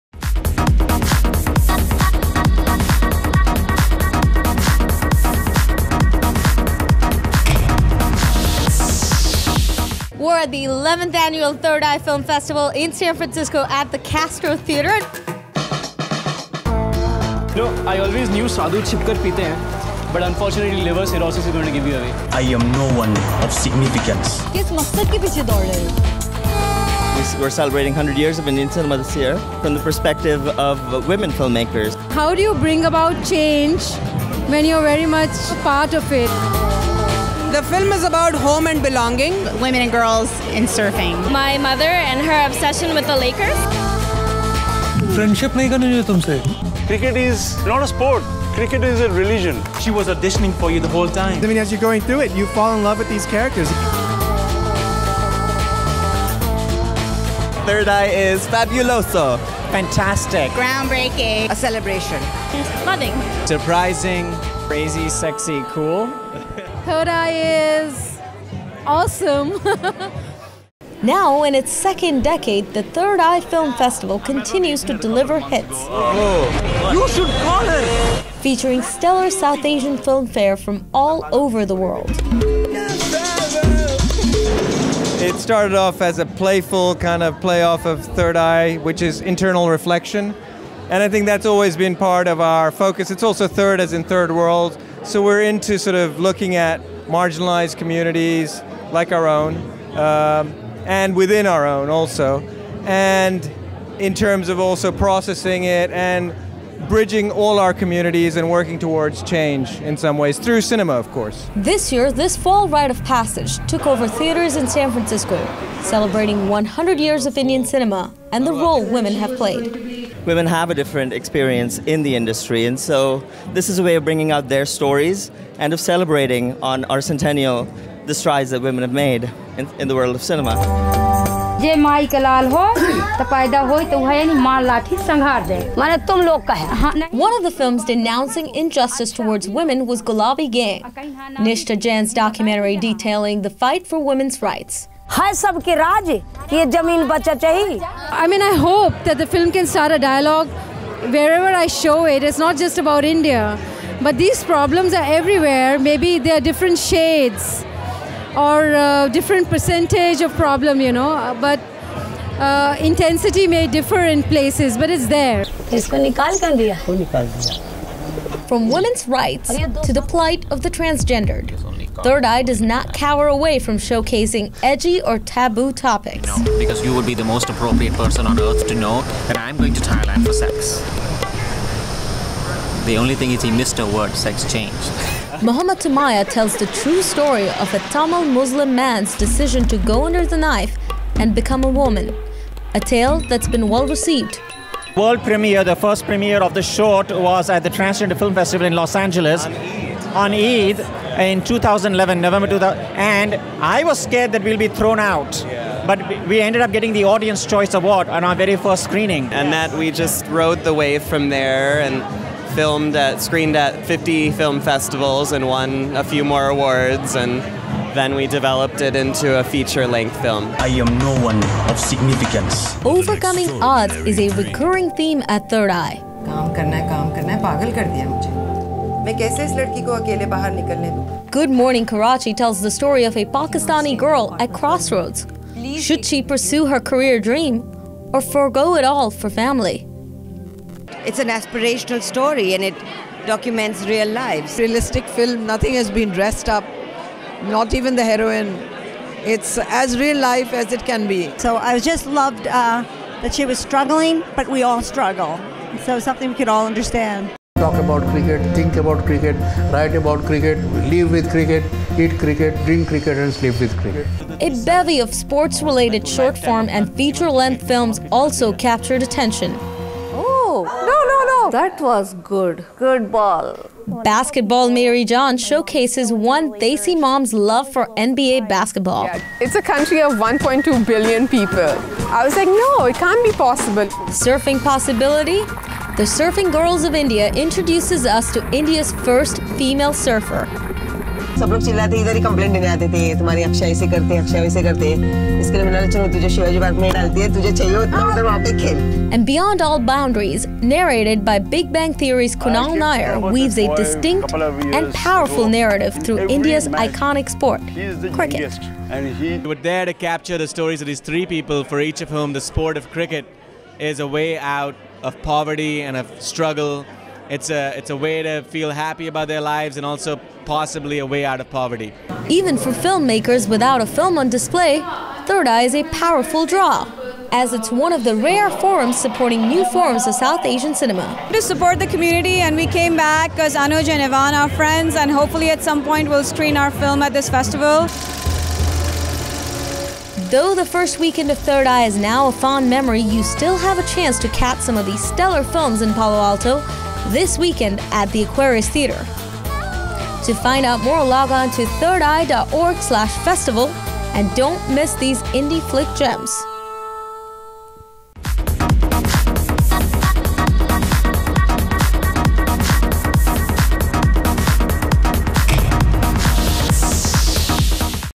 EXCLUSIVE: 3rdi South Asian Film Festival 2013
In its 11th year, the 3rdi South Asian Film Festival brought together a bevy of Indie flick gems. Diya TV spotlight brings you the exclusive report.

Find DiyaTV previews, promos, clips, and digital exclusives here.

Diya TV ®
Enlighten. Entertain. Inspire.™